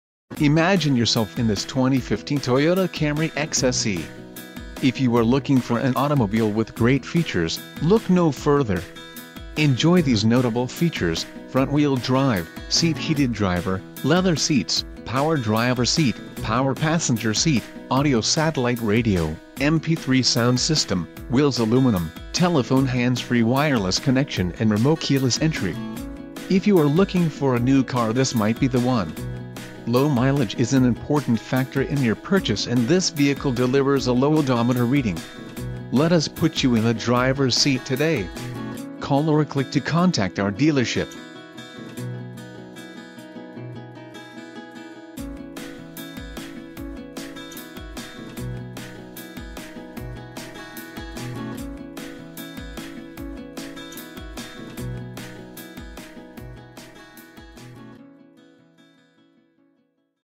2015 Toyota Camry XSE - Stadium Toyota - Tampa, FL 33614
Presenting our all-new 2015 Toyota Camry XSE in Cosmic Gray Mica. Powering our Camry is a spirited 2.5 Liter 4 Cylinder engine that pairs with a paddle-shifted 6 Speed automatic transmission and generates 178hp on demand. This efficiency rewards you with nearly 35mpg on the open road and the confidence of sport-tuned suspension.    The bold new exterior of the XSE is accented by 18 inch alloy wheels with a black-painted finish and a distinct front grille. The ergonomically designed interior features Ultrasuede heated seats and an Entune Audio Plus touchscreen with Bluetooth phone and audio connectivity as well as a sound system with steering wheel mounted controls. Confident handling, straightforward technology and lots of comfortable passenger space make this car an absolute delight to own.